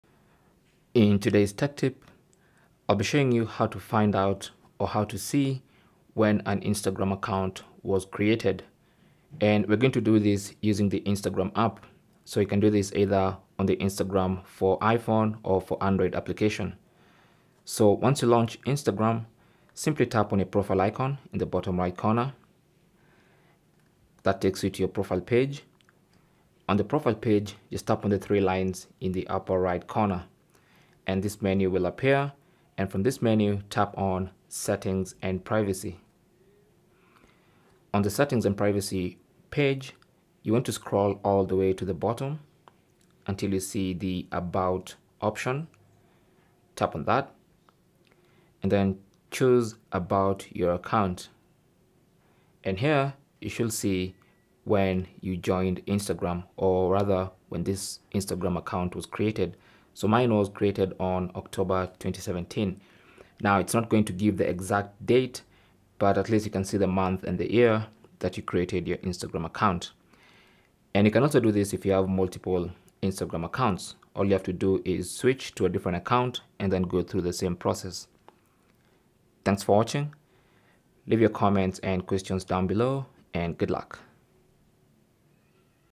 How To See When an Instagram Account Was Created
Learn How To See When an Instagram Account Was Created. In this video, we'll guide you through the simple steps to uncover the creation date of your Instagram account.

Whether you're curious about when you first joined Instagram or need this information for account recovery purposes, we've got you covered.

Remember, knowing your Instagram account's creation date can be a fun and insightful way to track your social media journey. Follow the steps in this video, and you'll have the exact date your Instagram adventure began!

1. Open the Instagram app and tap on your profile icon.

2. Tap on the three horizontal lines in the upper right corner to open a menu of options

3. From the options menu, select the Settings & Privacy option

4. From your Settings [age, scroll down and locate About. Tap on it.

5. Finally tap on About Your Account. You'll be taken to a page that shows you the date you joined

Cell Phone Tripod Adapter